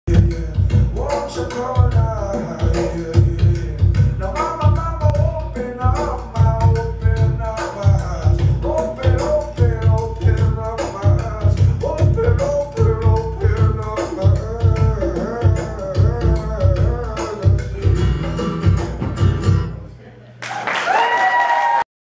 Yogi - Ram Dass Play, Ocean Beach (2014)
A man performs several Yoga postures at the Ram Dass play in Ocean Beach.